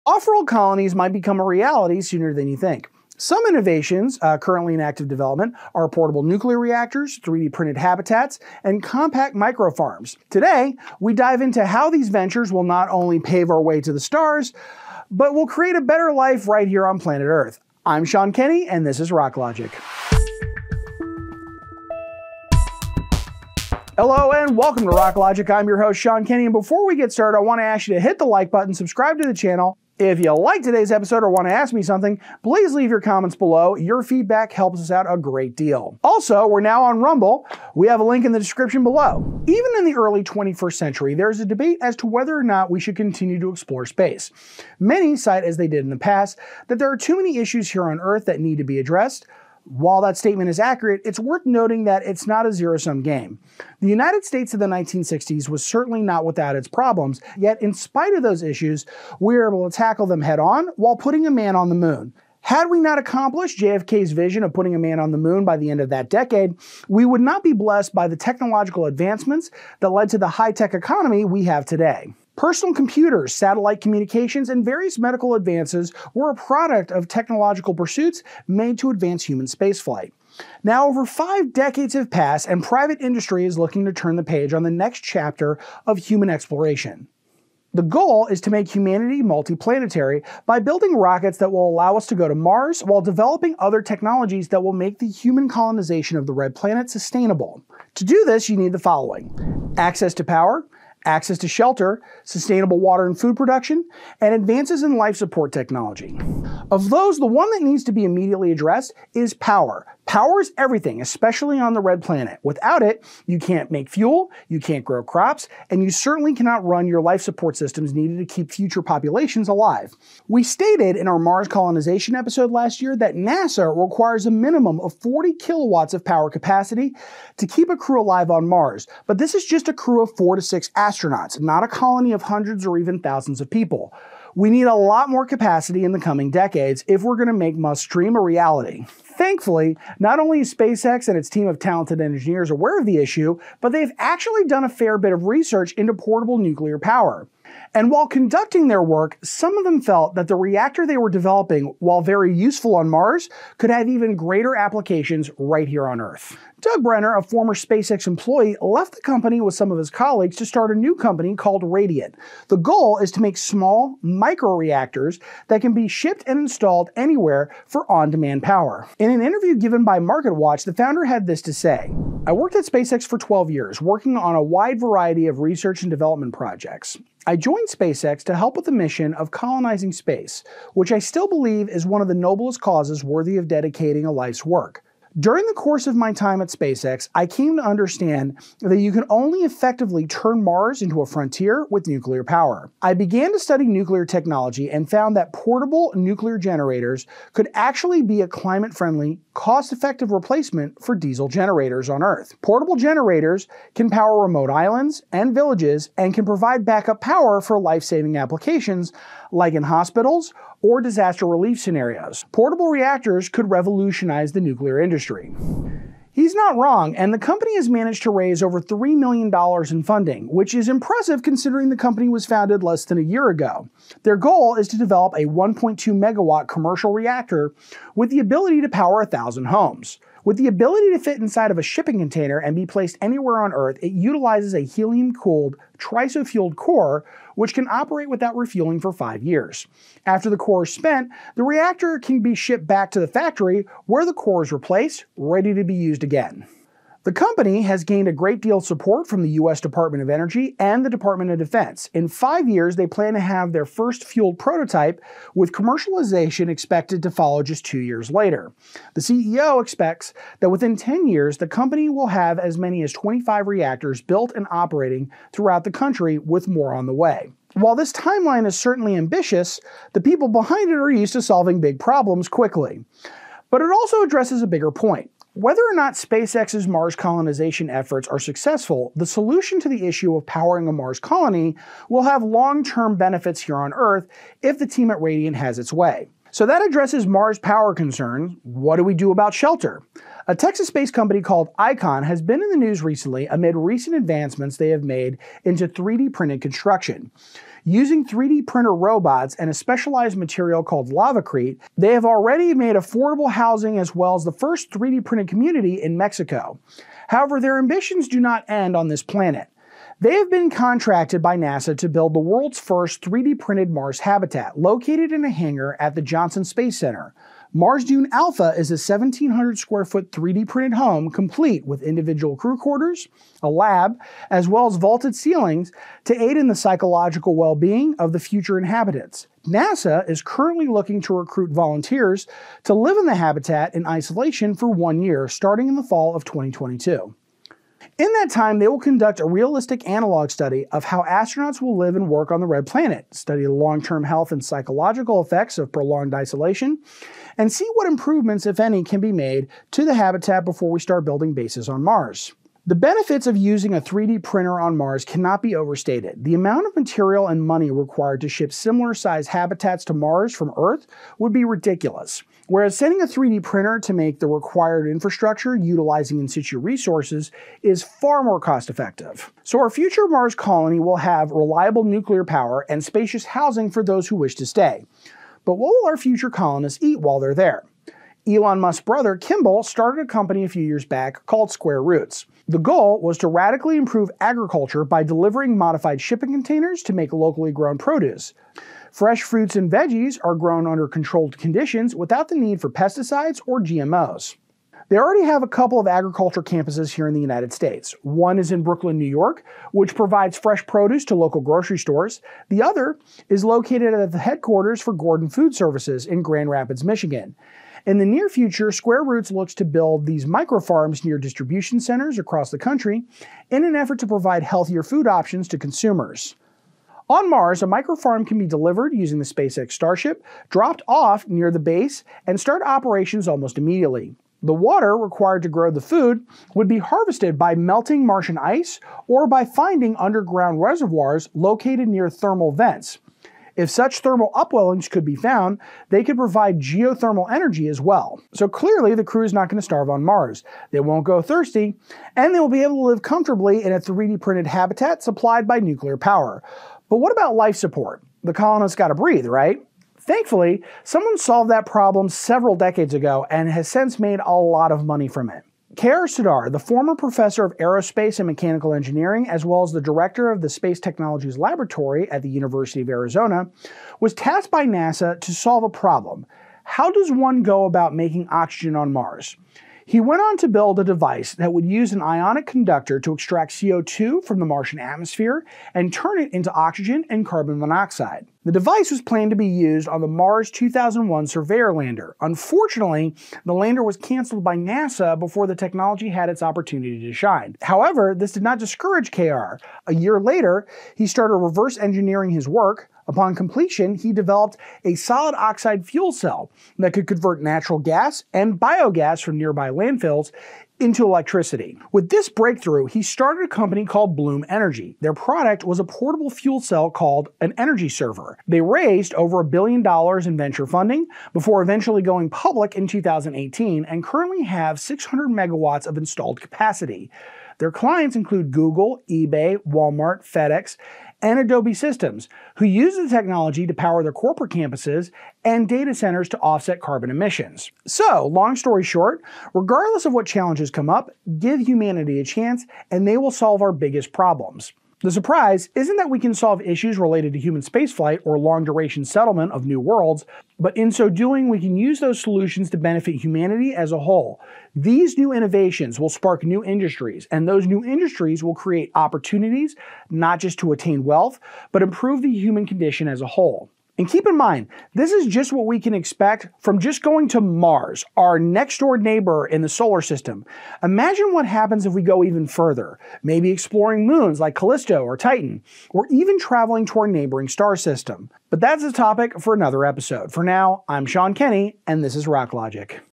Mars Colonization Tech | Portable Nuclear Power, 3D Printed Homes, Portable Farming | Ep 35
**Feb 2022 Update** Rock Logic now has merch for sale!

Elon Musk’s SpaceX wants to colonize Mars and is building reusable rockets to do so. But more than just new rockets are needed. Today, Sean Kenny discusses what types of technology are needed to be able to colonize outer space and beyond along with companies actively developing that technology as we speak.

0:00 Intro
Will Colonizing Space Help Humanity?
Elon Musk’s SpaceX Vision
Radiant Energy’s Answer to Powering a Mars Colony
Icon - 3D Printed Houses on Mars
Kimbal Musk’s Shipping Container Micro farms/Square Roots
Bloom Energy’s Life Support Systems for Mars

Want an audio-only experience on the go? Check us out on other platforms

Feeling generous? Every dime we get goes towards production and marketing.